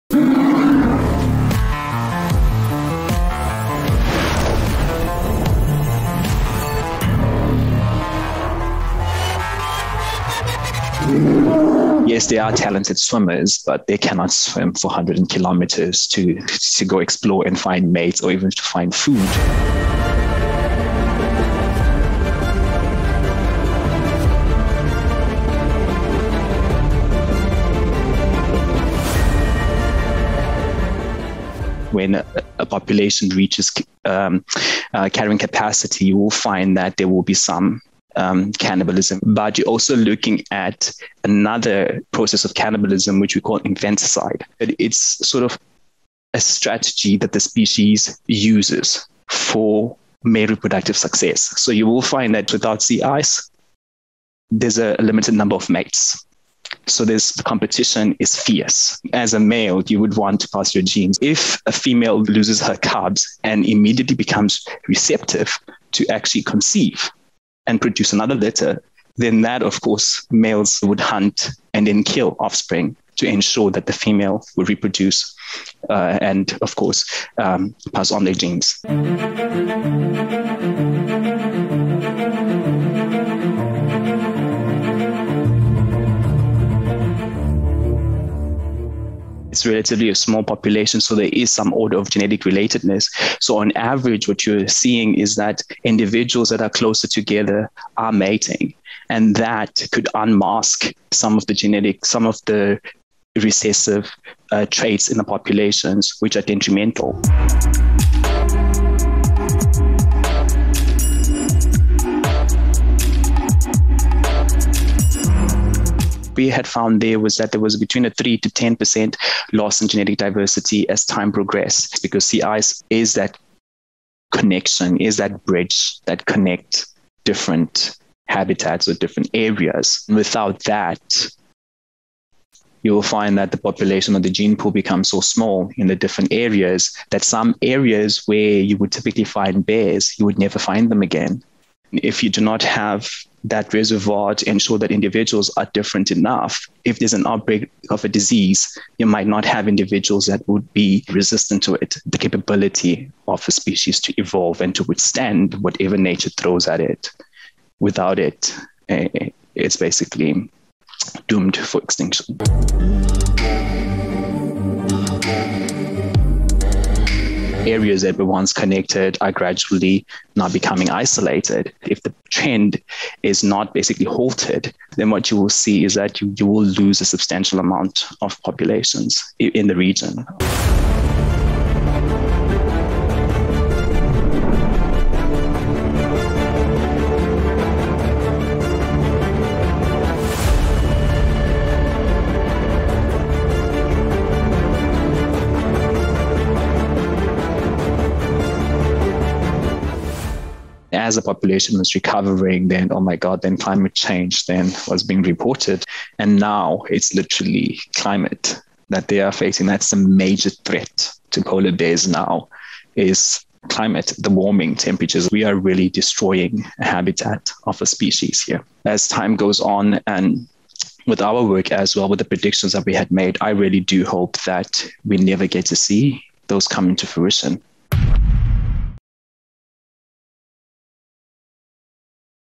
How Climate Change is Affecting Polar Bears
Another disturbing effect of climate change — polar bears are eating their cubs. This is why.